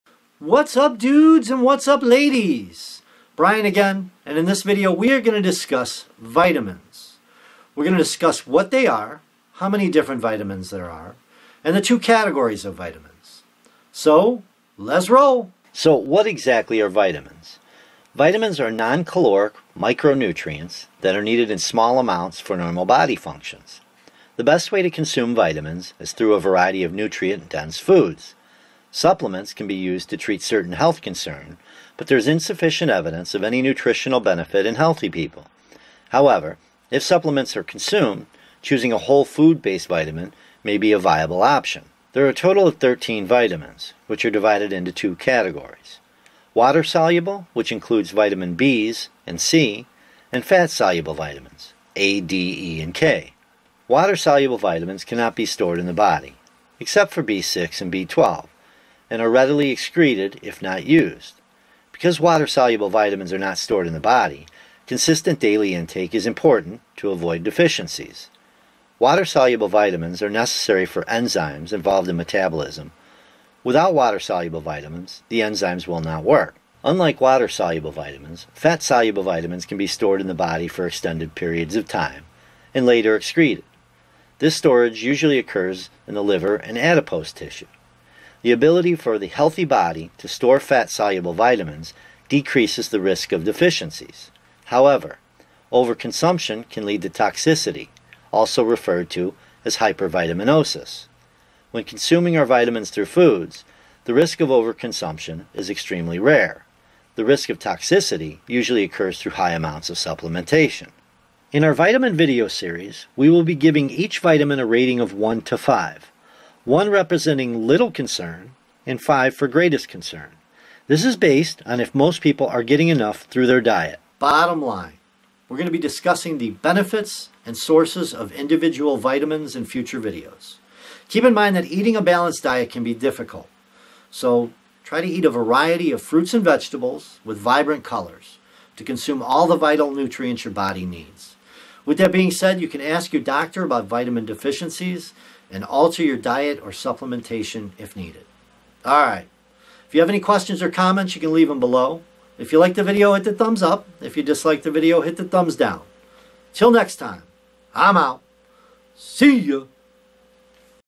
Vitamins - What are Vitamins - Types Of Vitamins - Fat Soluble Vitamins - Water Soluble Vitamins
In this video I discuss what are vitamins, what do vitamins do, and how many vitamins are there.  I also discuss the different types of vitamins, water soluble vitamins and fat soluble vitamins.

Transcript

Intro
Whats up dude, and whats up ladies? Bryan again, and in this video I am going to discuss vitamins.  We are going to discuss what they are, how many of them there are, and the 2 different categories of vitamins, so, lets roll.

So, what exactly are vitamins?  Vitamins are non-caloric micronutrients that are needed in small amounts for normal body functions.  The best way to consume vitamins is through a variety of nutrient-dense foods.  Supplements can be used to treat certain health concerns but there is insufficient evidence of any nutritional benefit in healthy people. However if supplements are consumed choosing a whole food based vitamin may be a viable option.

There are a total of 13 vitamins, which are divided into 2 categories, water- soluble which includes vitamin B’s and C; and fat -soluble vitamins A,D,E and K.

Water-soluble vitamins cannot be stored in the body, except for B6 and B12, and are readily excreted if not used. Because water-soluble vitamins are not stored in the body, consistent daily intake is important to avoid deficiencies. Water –soluble vitamins are necessary for enzymes involved in metabolism, without water soluble vitamins the enzymes will not work.

Unlike water-soluble vitamins, fat-soluble vitamins can be stored in the body for extended periods of time and later excreted. This storage usually occurs in the liver and adipose tissue..  The ability for the healthy body to store fat-soluble vitamins decreases the risk of deficiencies however, over consumption can lead to toxicity, also referred to as hypervitaminosis.

When consuming our vitamins through foods, the risk of over consumption is extremely rare. The risk of toxicity usually occurs through high amounts of supplementation.

In our vitamin video series, we will be giving each vitamin a rating of 1 to 5. 1 representing  little concern  and 5 for greatest concern. This is based on if most people are getting enough through their diet.

Bottom line: We will discuss the benefits and sources of individual vitamins in future videos. Keep in mind that eating a balanced diet can be difficult. But, try to eat a variety of fruits and vegetables with vibrant colors to consume all of the vital nutrients your body needs. With that said ask your doctor about any possible vitamin deficiencies and alter your diet/supplementation if needed. Comments and questions…like or dislike below.

Timestamps
0:00 Intro
0:15 What are vitamins?
0:42 Types of vitamins - water soluble and fat soluble
0:54 Water soluble vitamins
1:16 Fat soluble vitamins
2:11 Vitamin consumption